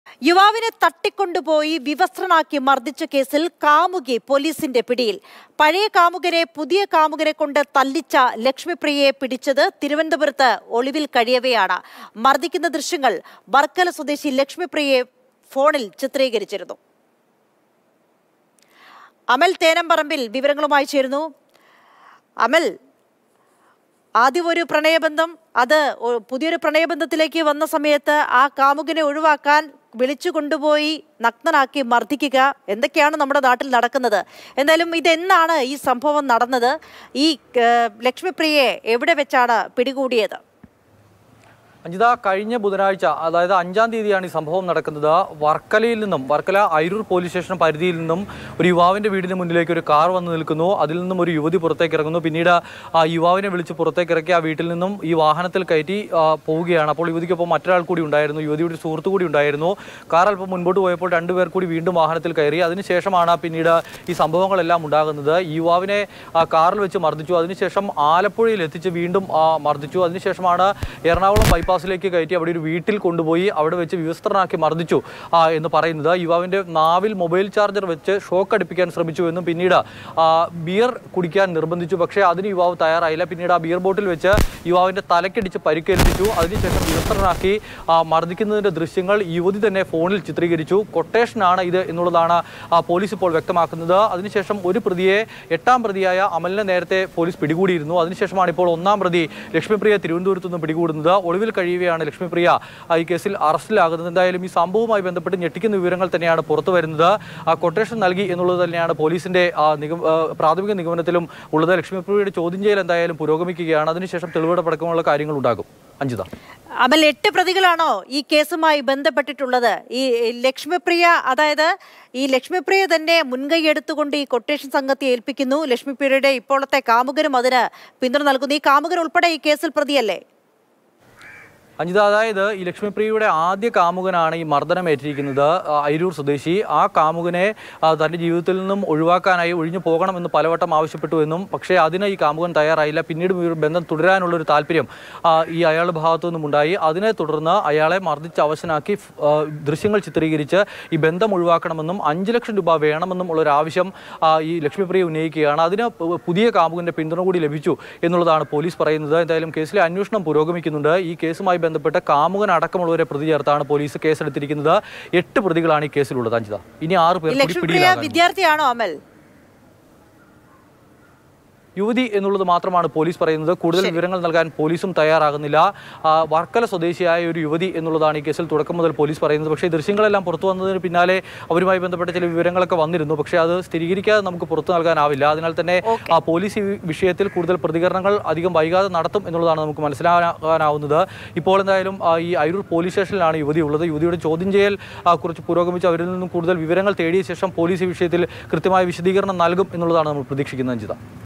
പഴയ കാമുകനെ പുതിയ കാമുകനെ കൊണ്ട് തല്ലിച്ച് കാമുകി; വര്‍ക്കല സ്വദേശി ലക്ഷ്മിപ്രിയ പിടിയില്‍
Happy viewing!